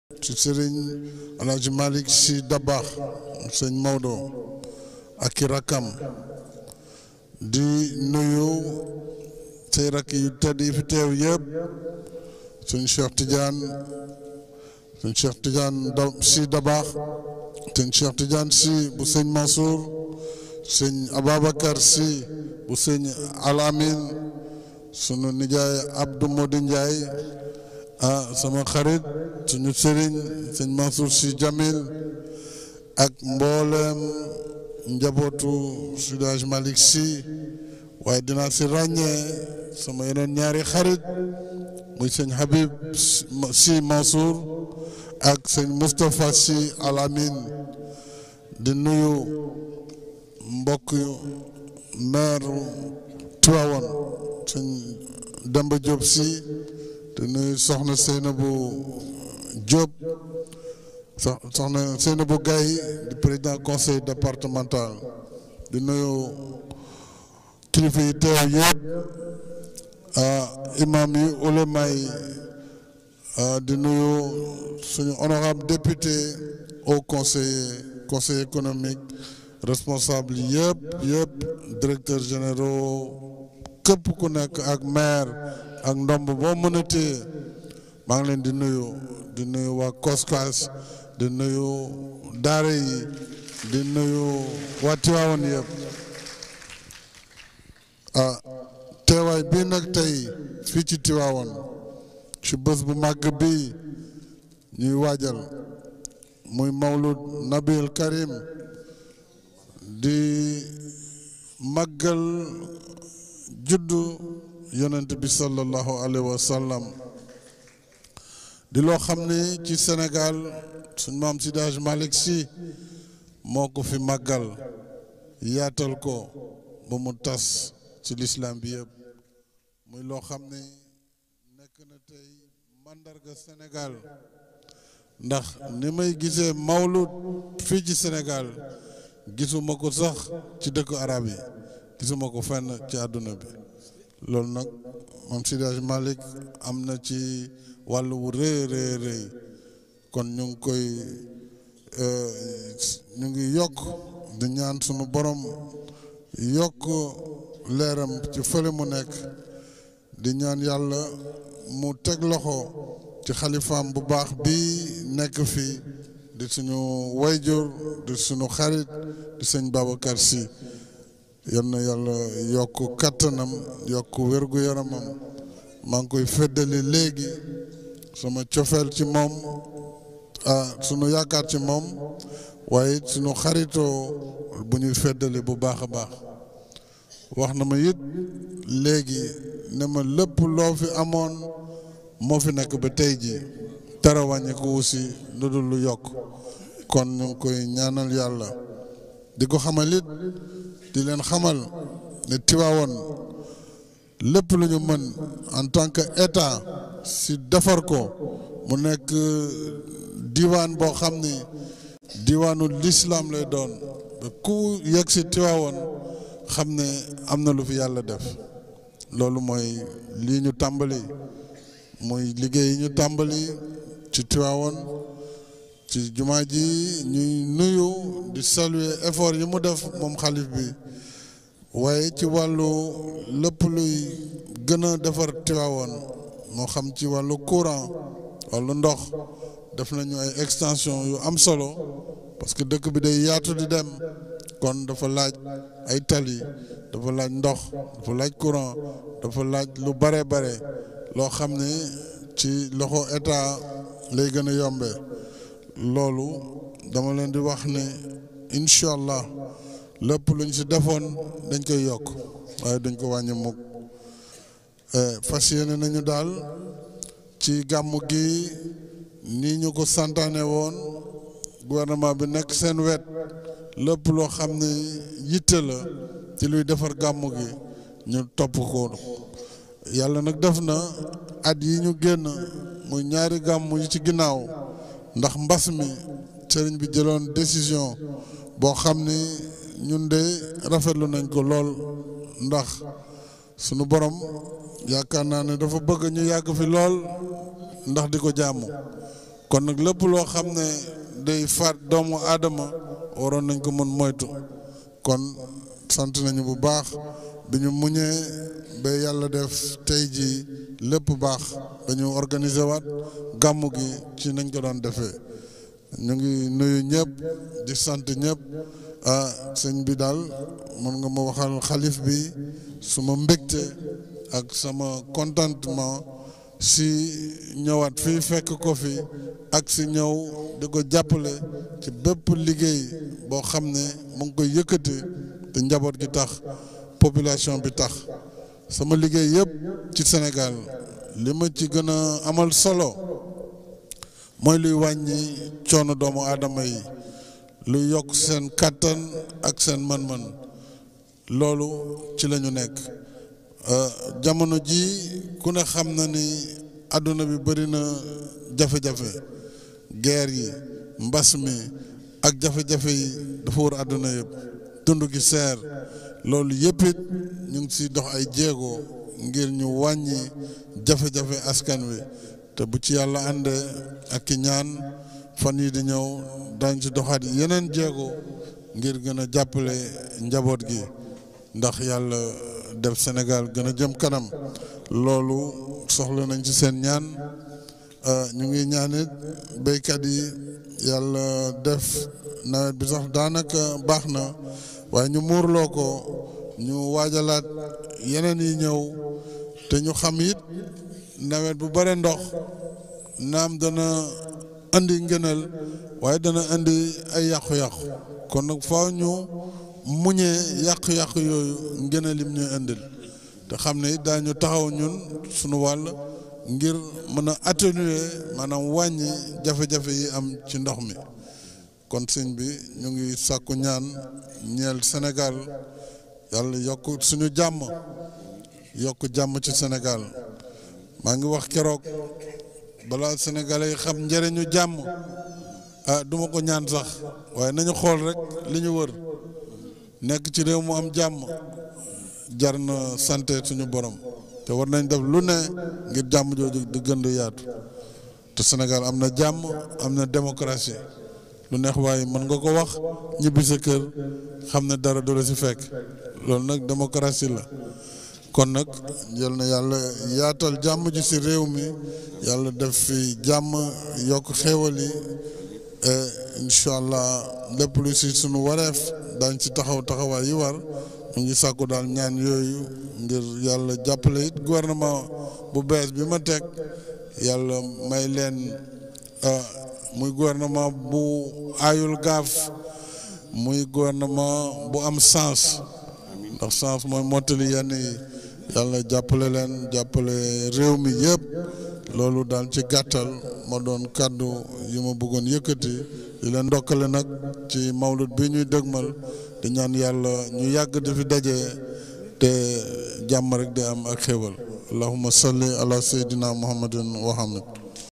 🛑 VISITE DU PRESIDENT MACKY SALL A TIVAOUNE IMPORTANT DISCOURS GAMOU 2022 ...
EDBTV est une chaine qui donne de l'information en tmps réel Politique ,Sport ,Culture ,Religion